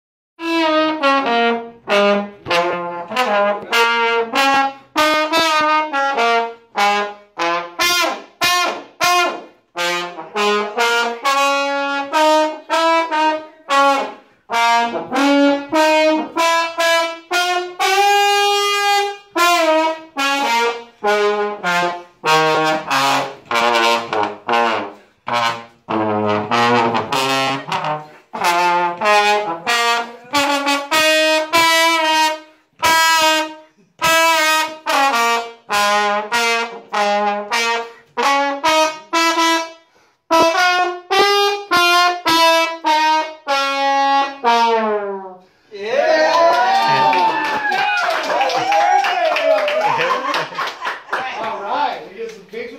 Trombone Vid
How does one go from playing trombone in Harlem to running three repair shops in Arizona? Well, let’s see: Frank Leutz grew up in Harlem and became a jazz salsa musician accomplished enough to play Carnegie Hall. He joined the Navy doing mine sweep operations in the Persian Gulf until he was transferred west. Then he drifted into auto repair after getting a job at a Chevy dealership, met and married his wife, who encouraged him to go independent, and opened the first of his Desert Car Care Service Centers in 2004.